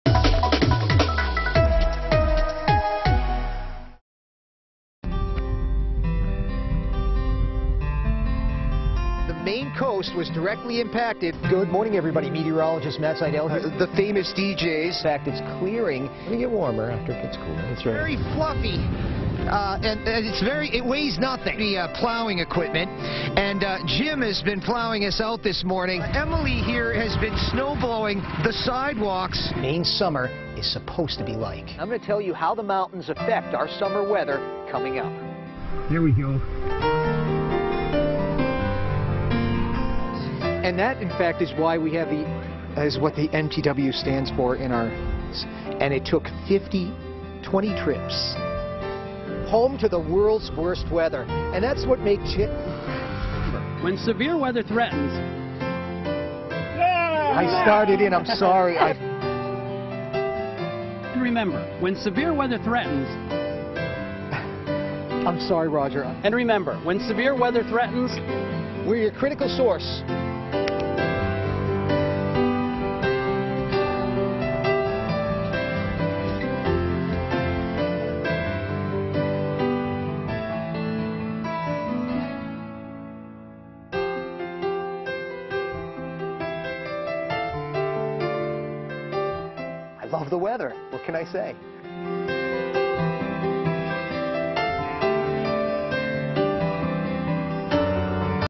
Matt Zidle says goodbye to WMTW
For more than a decade, Meteorologist Matt Zidle has brought you Maine's Total Weather on WMTW News 8 this morning. On Friday, we said goodbye as Matt moves on to his next journey as a teacher.